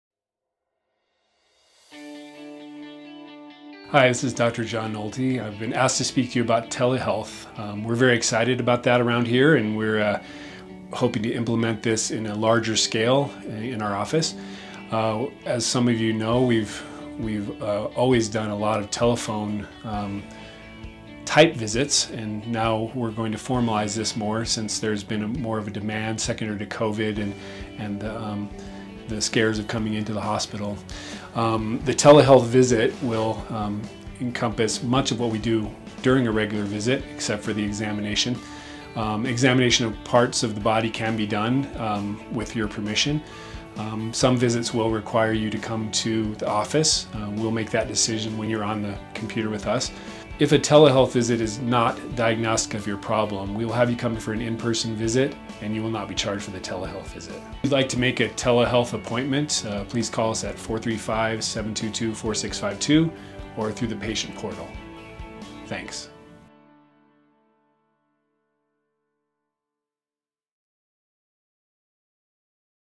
Telehealth For OB/GYN Care - Uintah Basin Healthcare
For many types of visits, telehealth allows our patients to receive care from the comfort of their own homes.